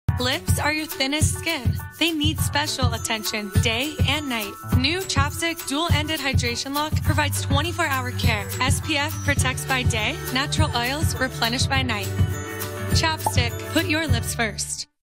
TV Commercial - ChapStick Duel Ended Hydration Lock - Featuring Alex Morgan - Put Your Lips First
Advertiser: ChapStick